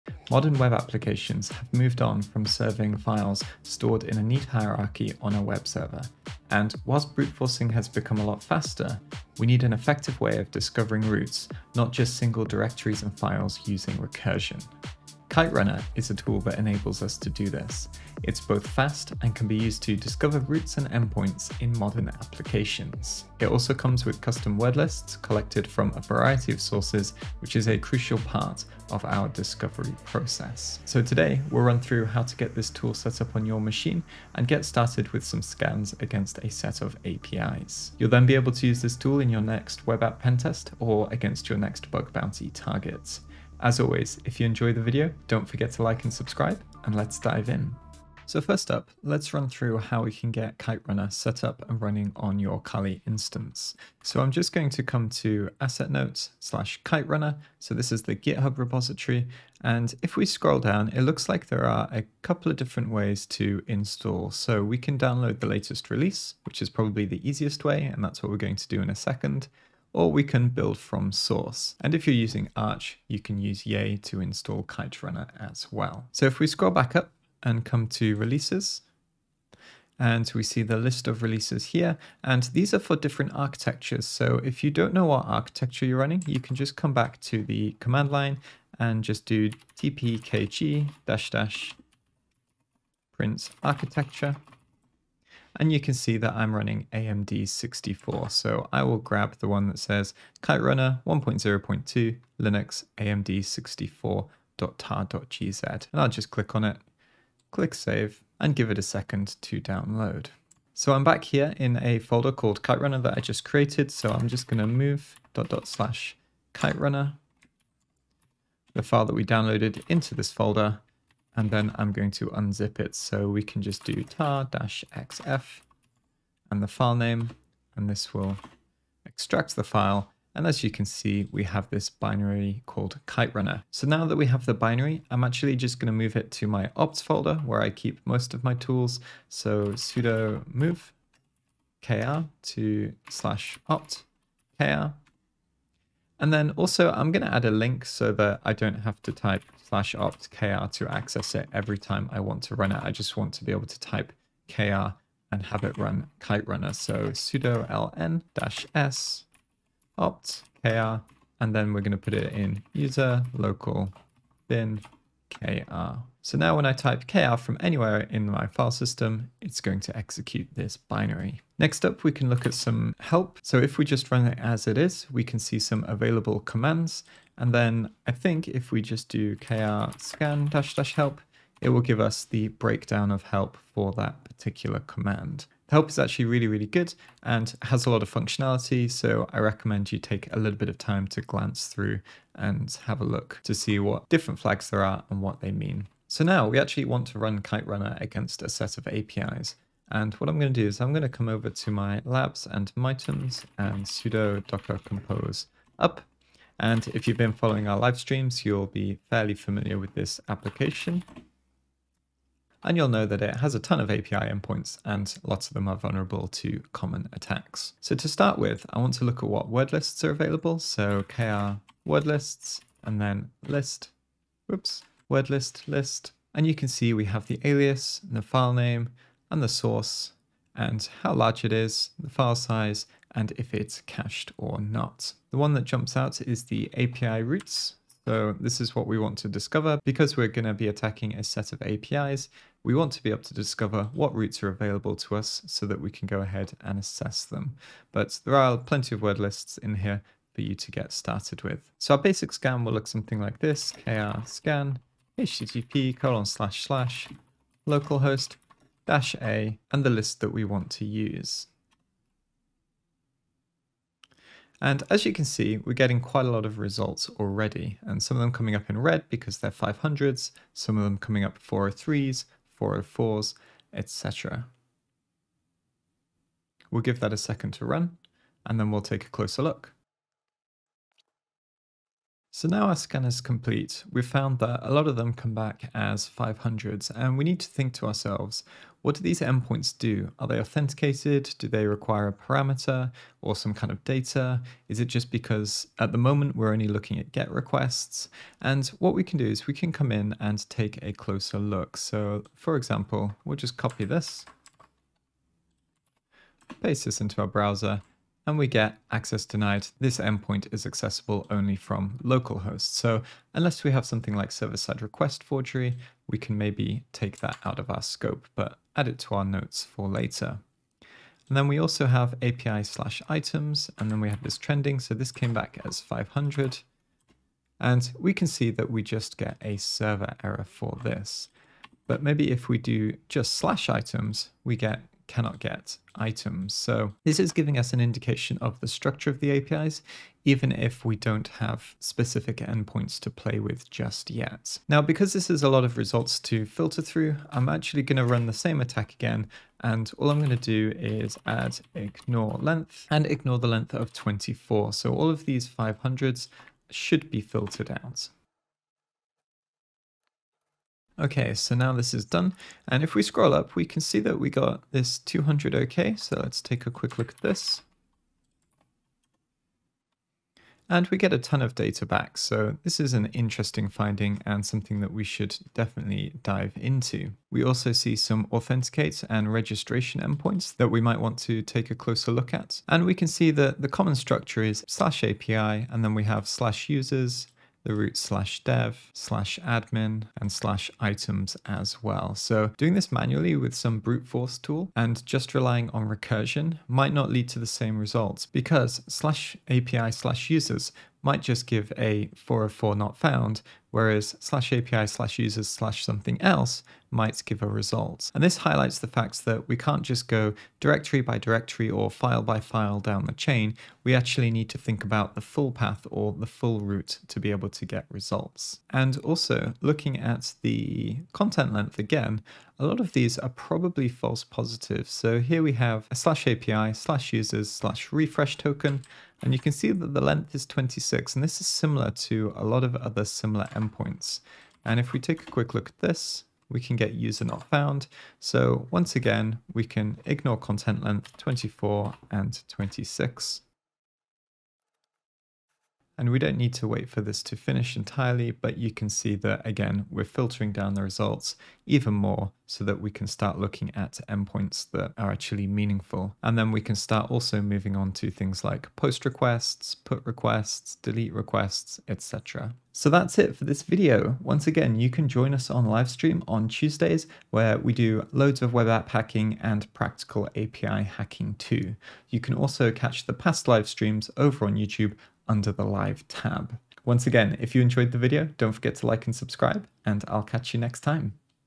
Next Level API Hacking with Kiterunner
00:00 Intro
00:47 Setup
02:52 Running kr
03:40 Wordlists
04:12 Basic scans
05:45 Filtering results
07:42 Outro